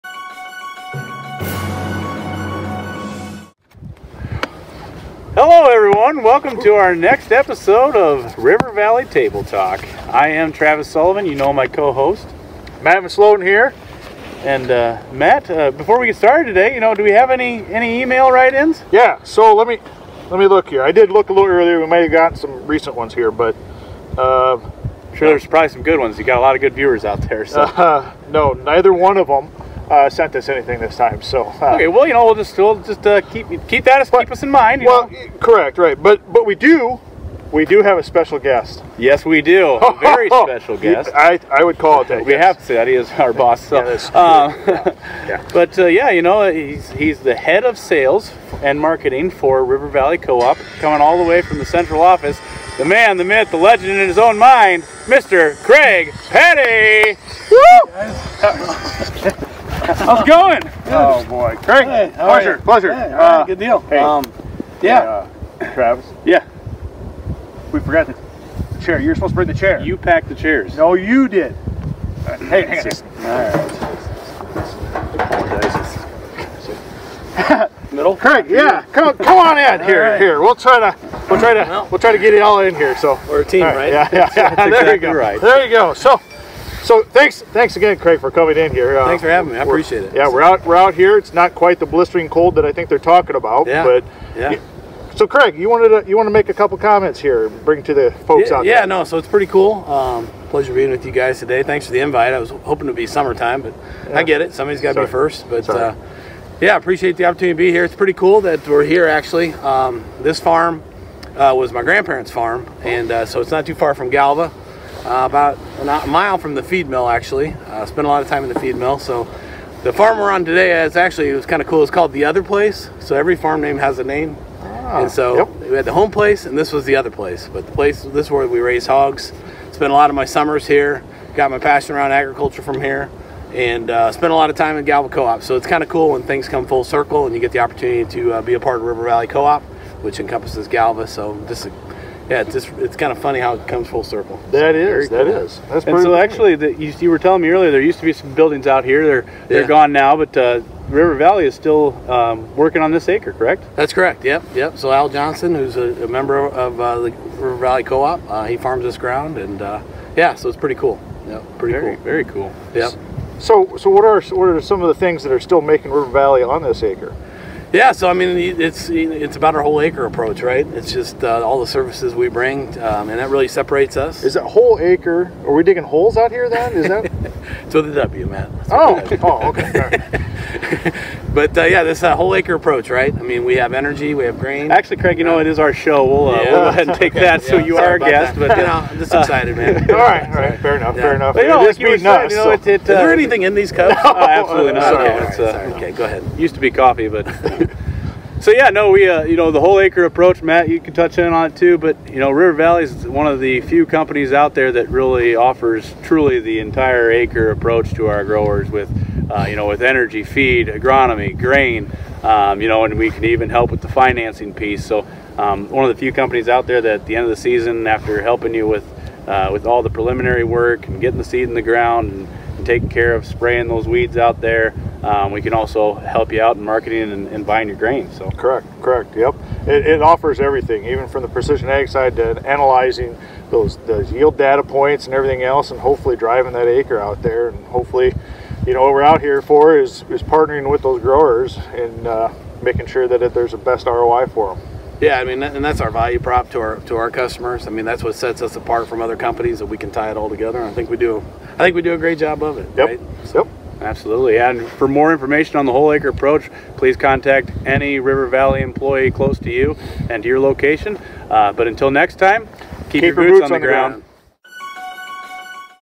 RVC Table Talk | Ep.4 | Whole Acre at RVC
🗣In our latest episode of Table Talk, special guest, Craig Patty, makes an appearance to visit about the Whole Acre Approach!

🌽 As you gear up for spring, learn how River Valley Cooperative can help you fine-tune your plant health management plan for 2020!